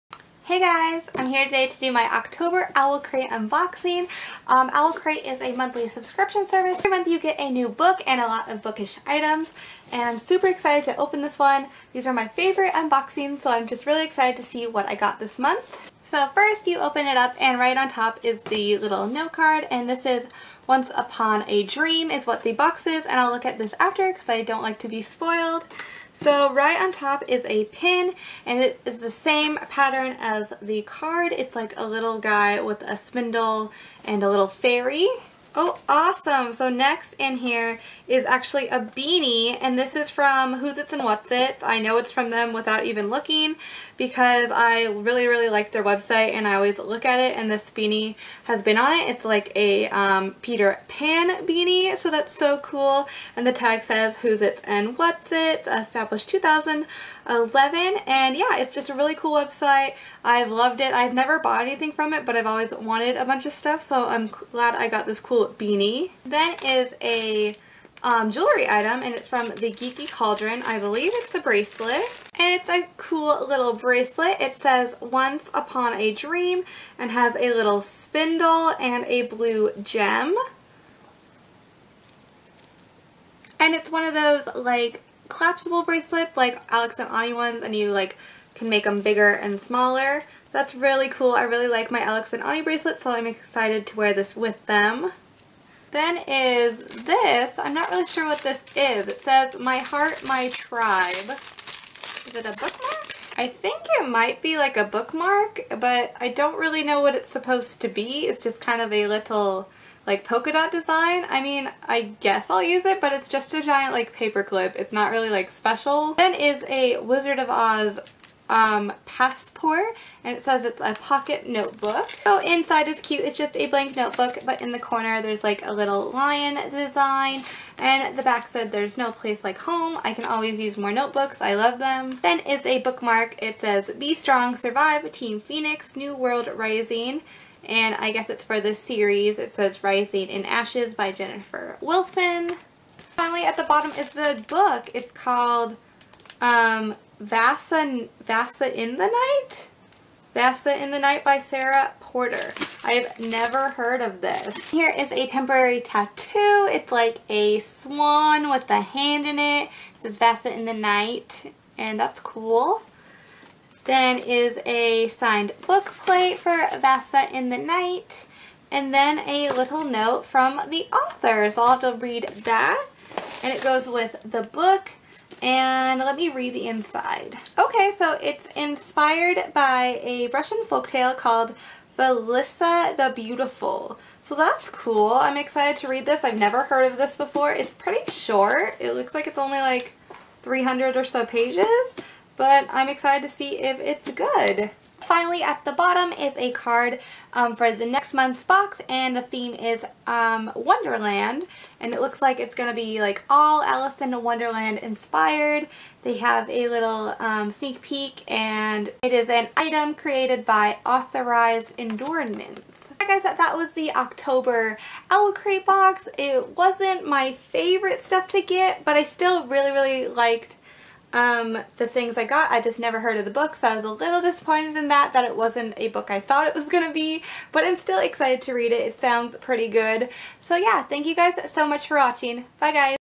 OwlCrate October 2016 Unboxing!
Hey guys! I love OwlCrate, and I hope you enjoy seeing what was in this months box!